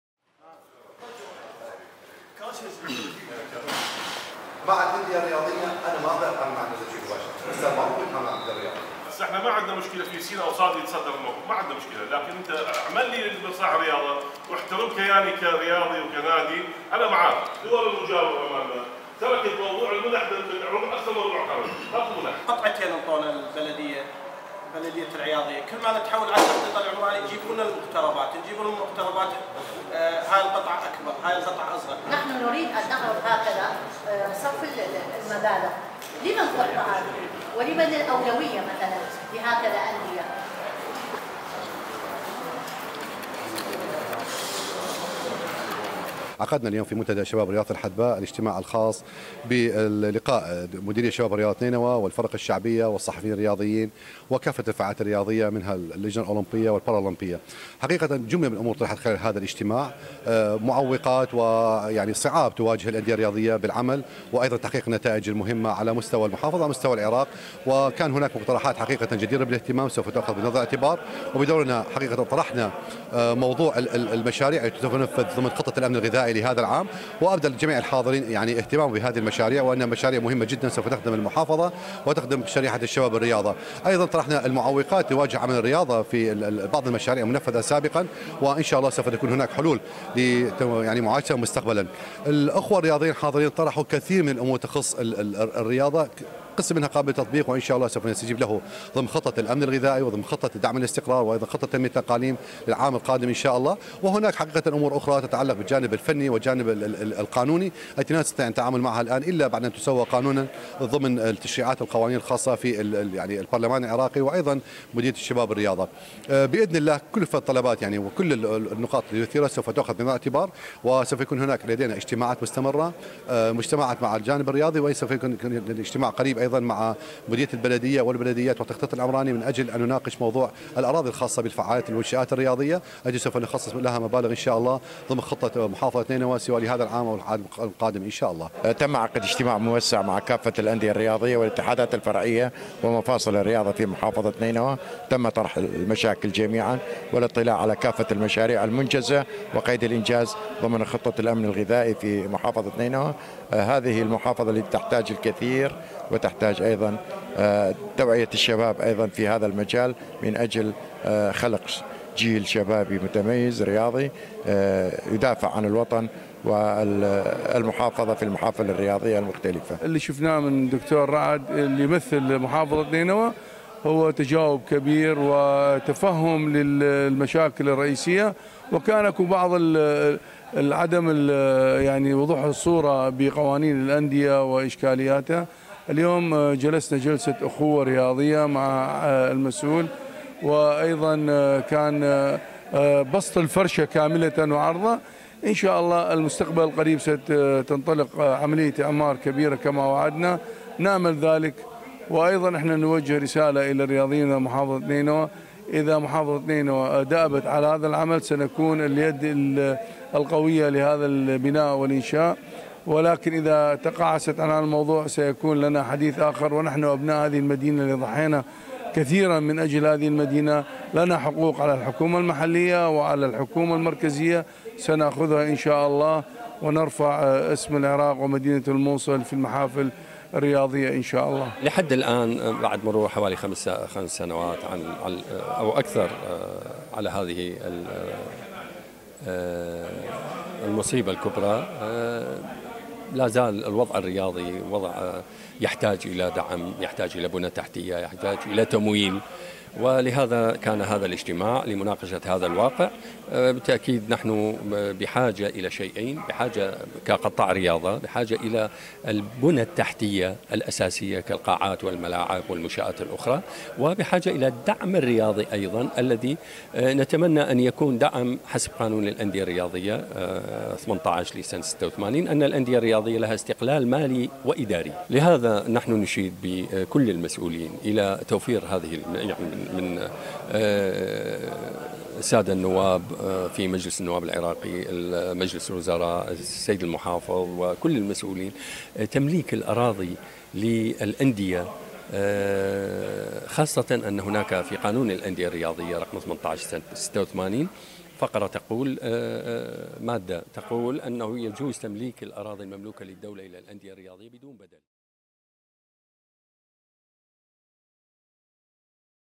العباسي يشرف على اجتماع ضم كافة الاندية والفرق المحلية والاتحادات الفرعية بنينوى
العباسي يشرف على اجتماع ضم كافة الاندية والفرق المحلية والاتحادات الفرعية بنينوى ويبحث معهم مشاكل القطاع الرياضي وتقديم الدعم لهم